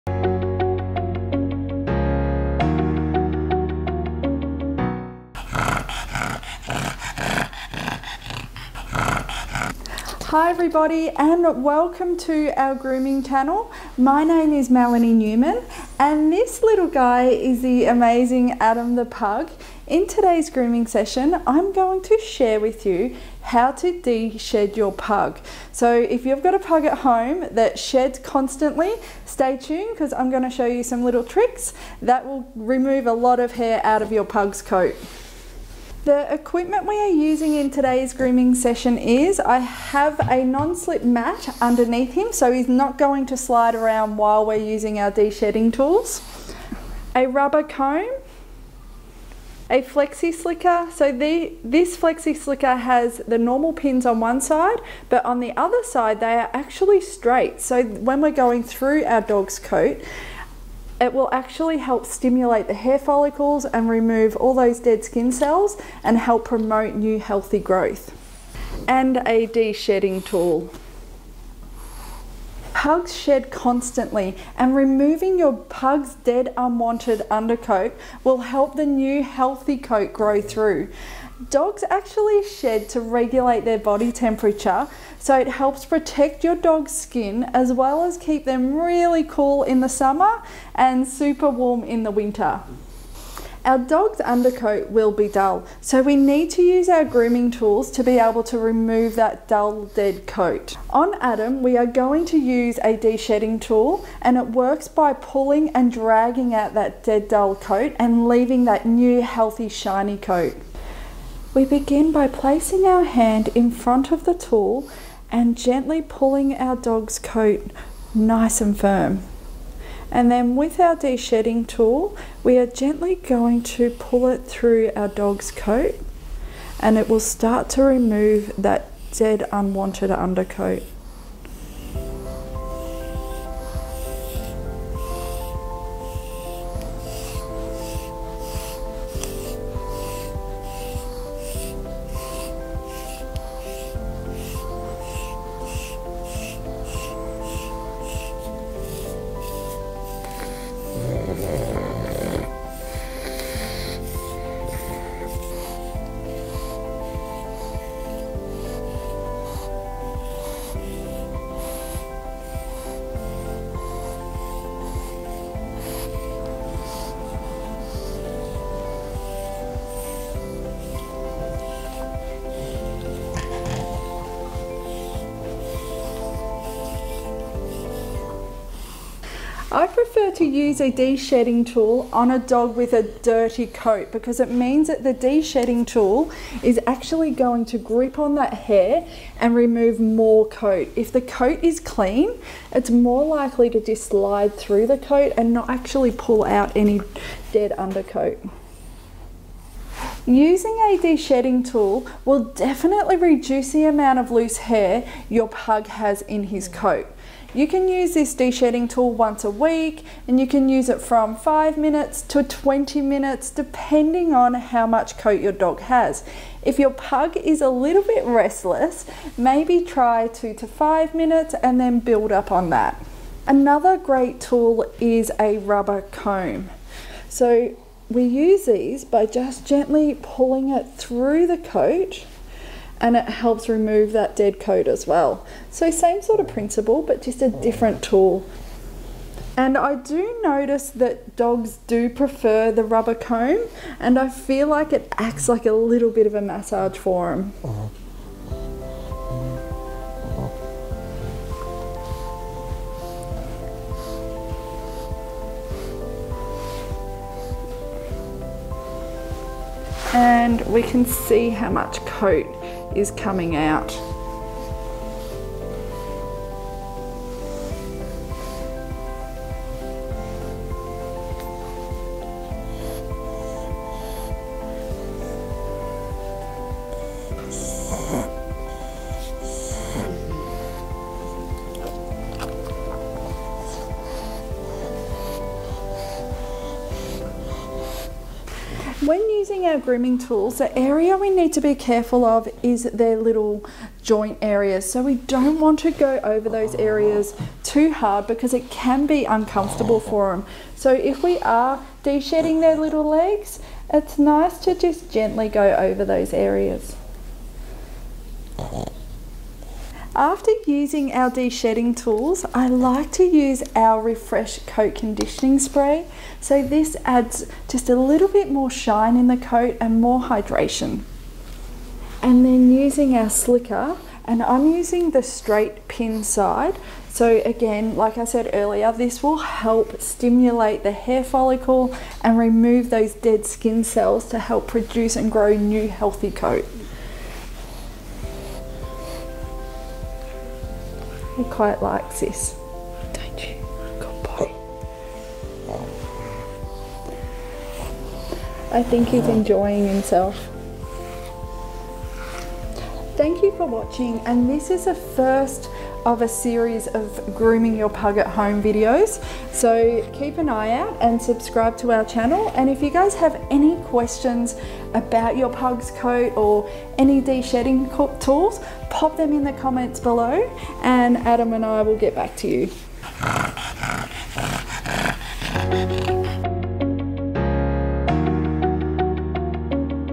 How to de shed a pug
This video Mel and Adam the wonderful Pug demonstrate how to de shed a Pug. Pugs loose a lot of hair and the more you can remove with a brush and during the bathing and drying process the less hair you will have in your house or car.
This is Part 1 of a 4 part How to groom your Pug series, we hope you enjoy them :-)